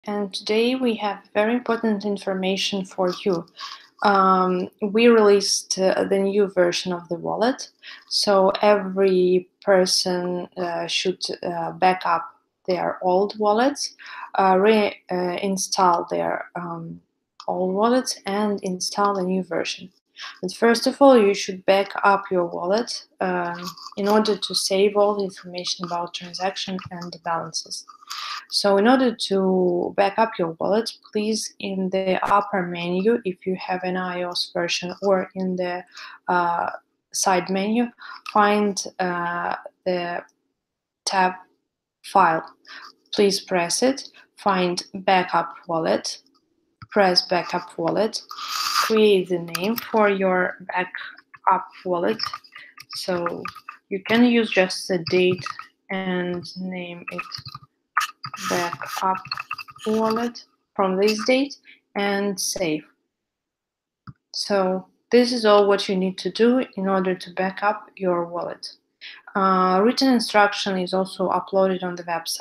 Octoin wallet backup
Octoin coin wallet got the core update. To save your funds from old wallet version please back up it.
watch the tutorial